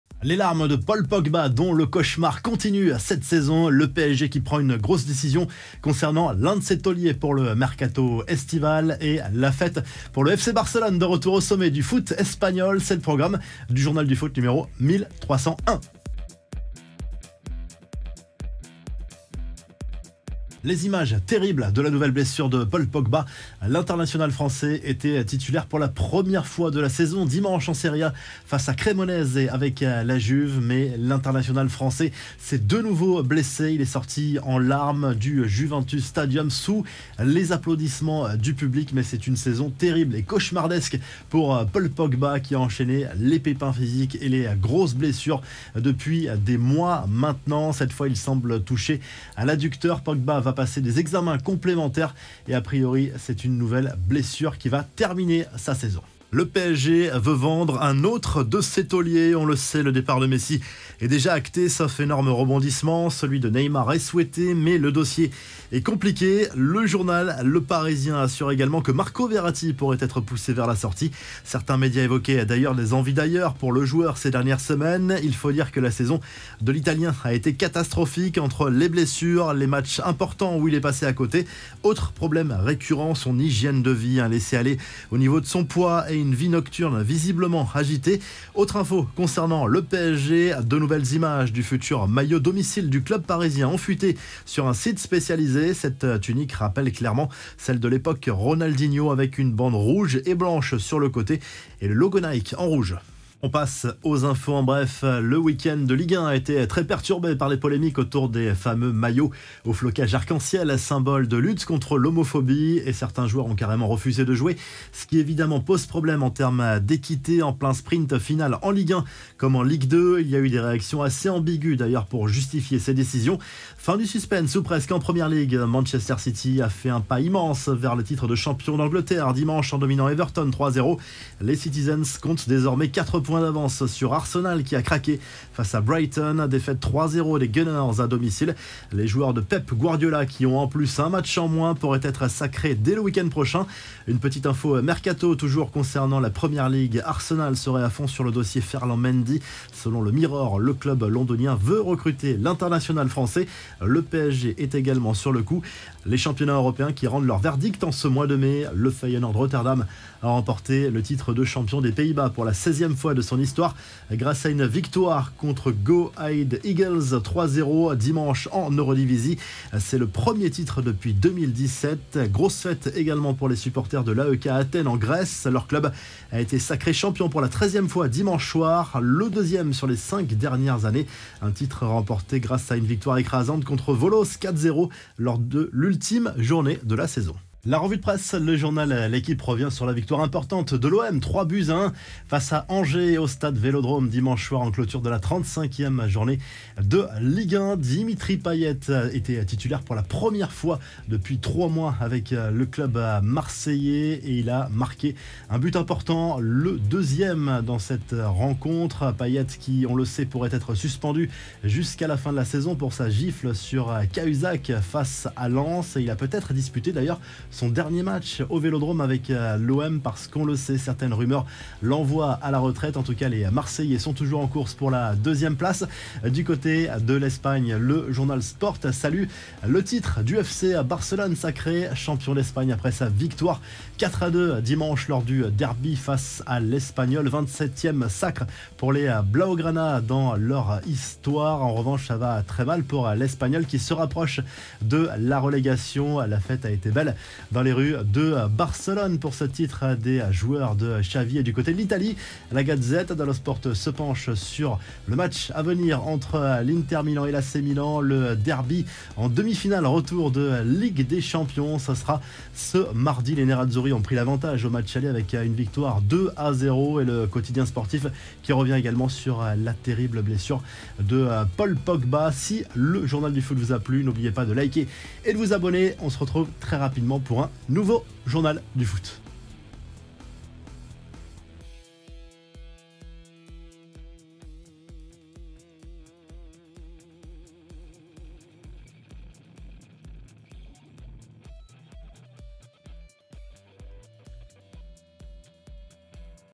MERCATO PSG : un INDÉSIRABLE de plus, le nouveau maillot a fuité ! POGBA : le cauchemar continue...
MERCATO PSG : un INDÉSIRABLE de plus et le nouveau maillot a fuité ! Paul POGBA : le cauchemar continue, la revue de presse... Le journal du foot et du mercato N°1301 est en ligne !